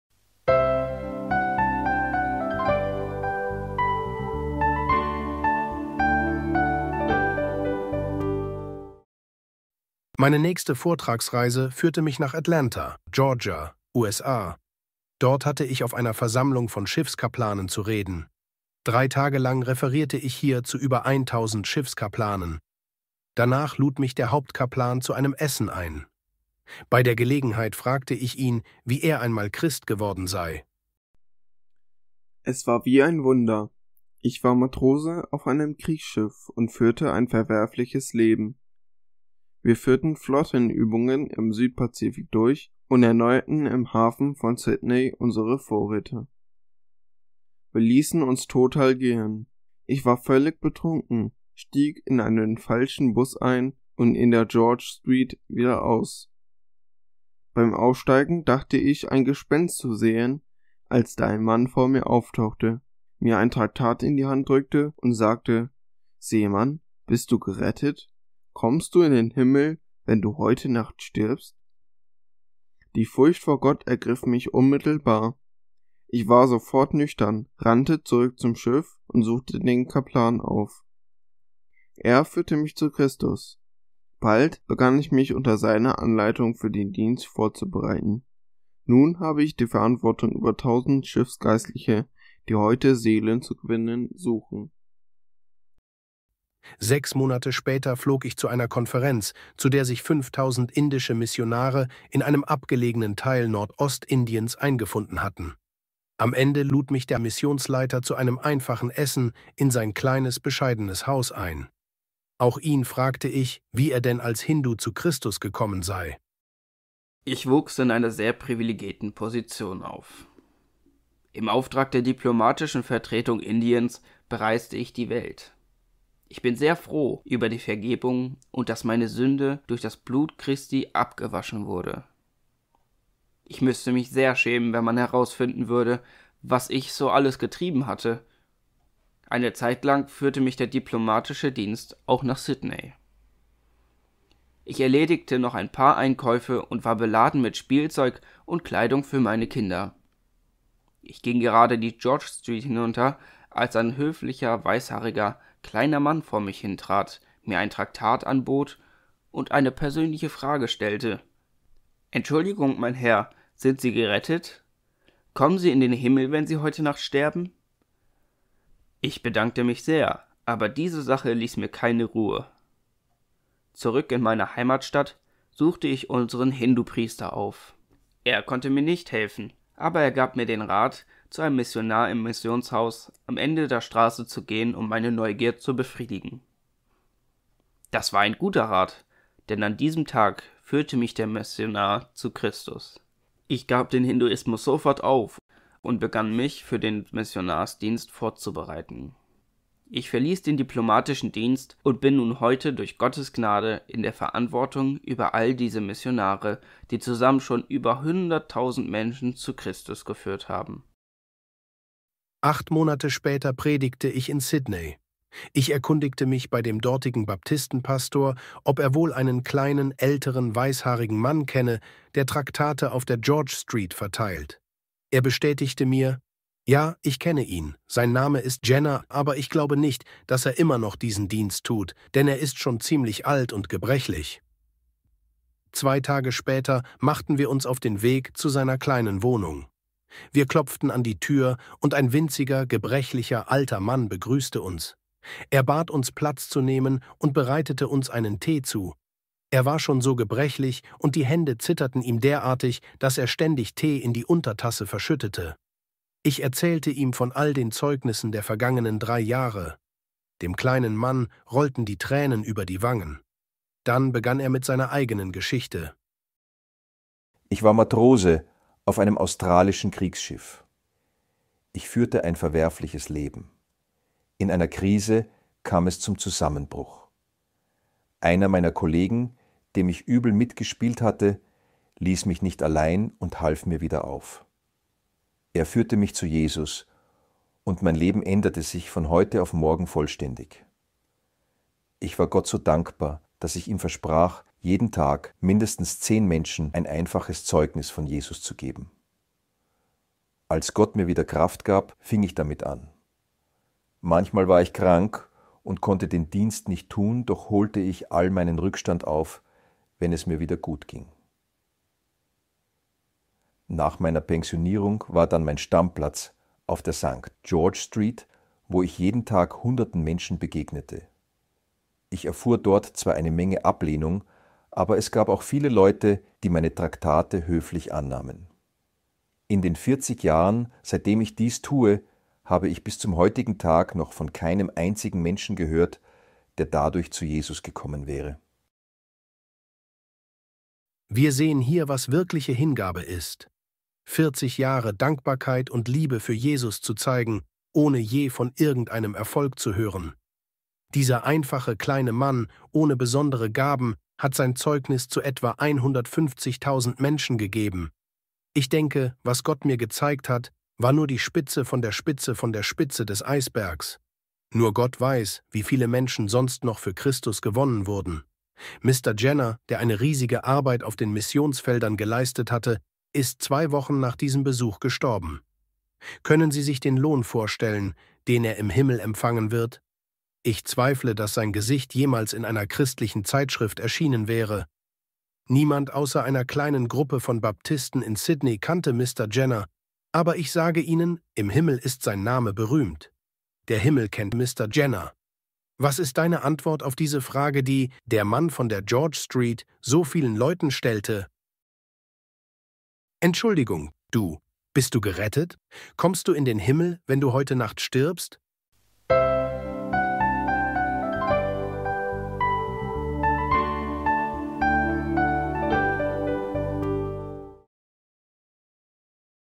Der kleine Mann von der George Street - Teil 2
Hör dir die beeindruckenden Erlebnisse an, die  Pastor Dave Smethurst aus London berichtet. - Teil 2